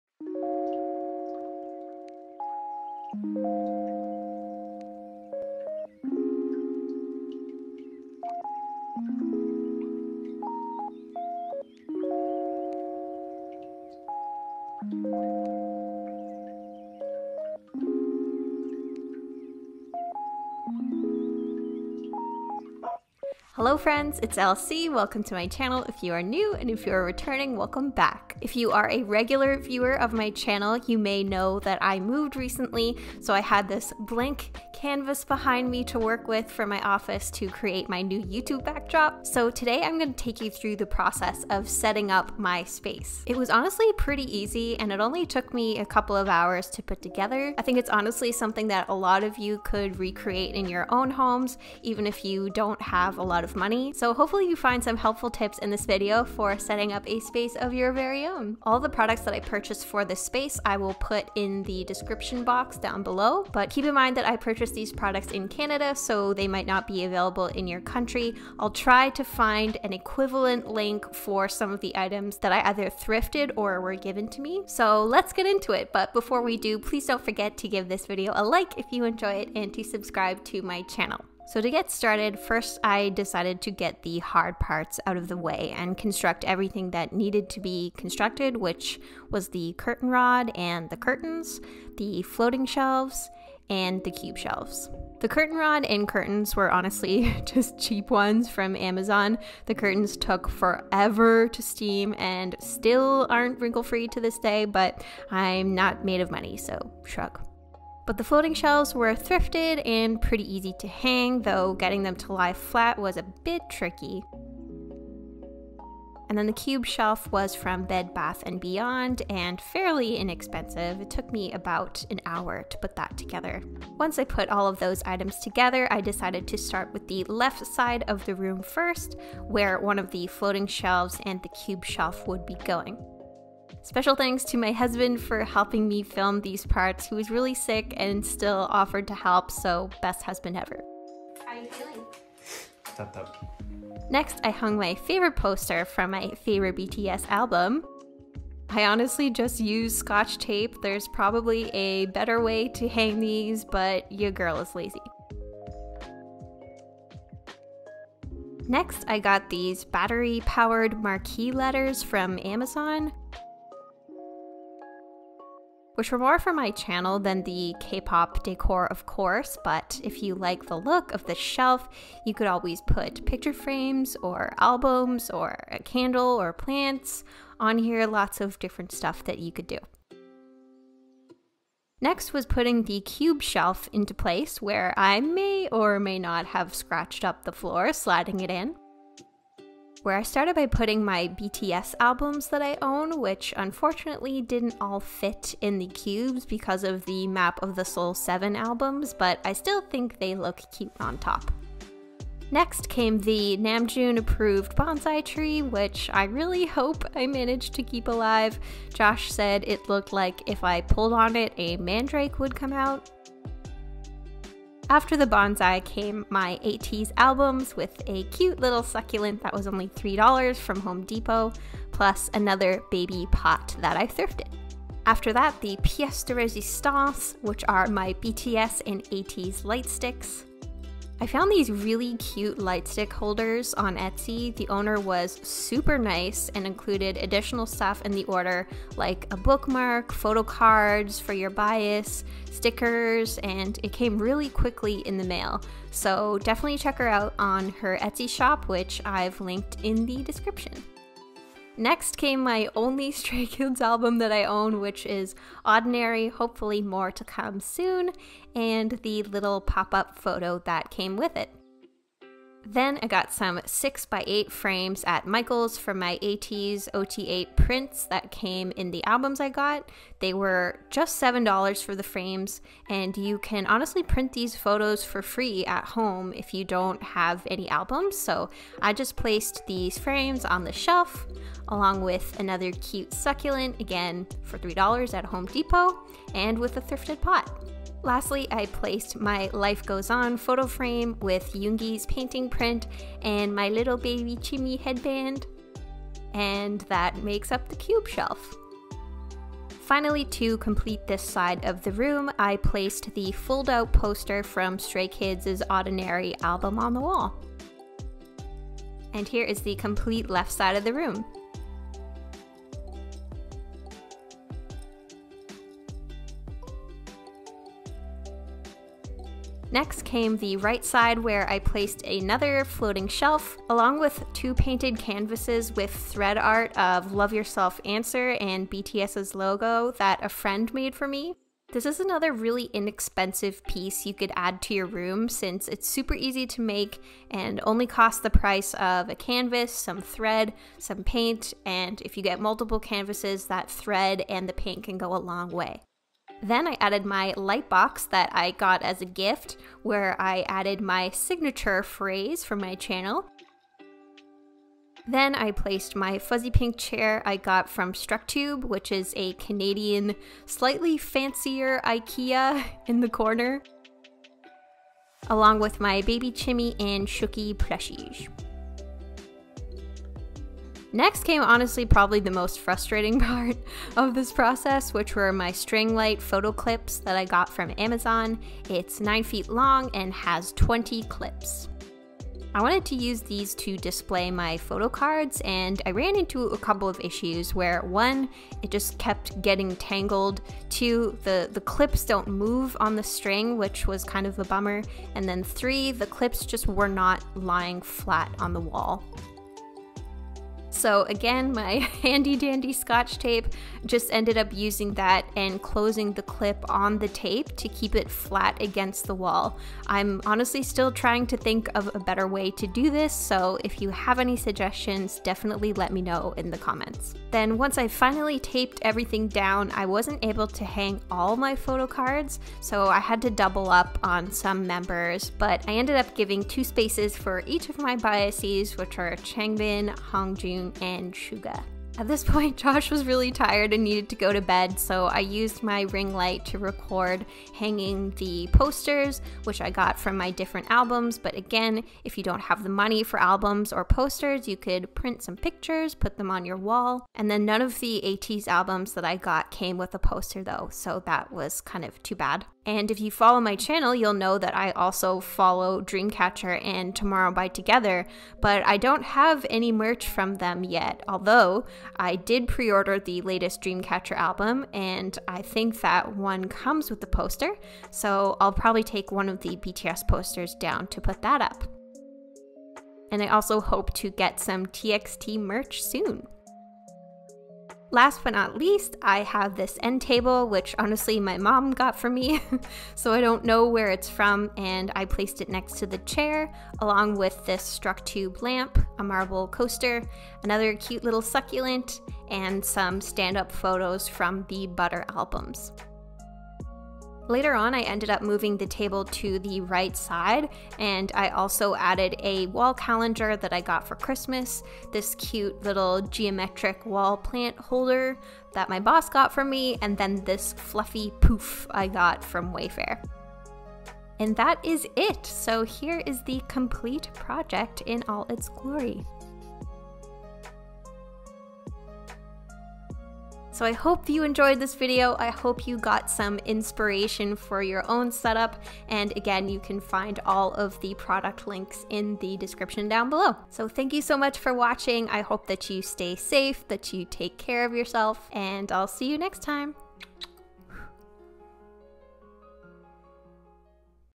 K-Pop Room Makeover | Easy & Simple DIY Decor
Easy and Simple K-pop Decor DIY Room Makeover Tutorial!

Today I’m taking you through the process of my simple and easy DIY K-pop decor room makeover for my YouTube backdrop! Hopefully you find some helpful decor tips for setting up a k-pop room makeover of your own.

Get in touch with me: laurenclaireyt [at] gmail [dot] com

PRODUCTS:

Floating shelves (similar)

Curtain rod

Curtains (white, 52 x 63)

Marquee letters

Light stick holders

Photo clip lights

Wall planter (similar)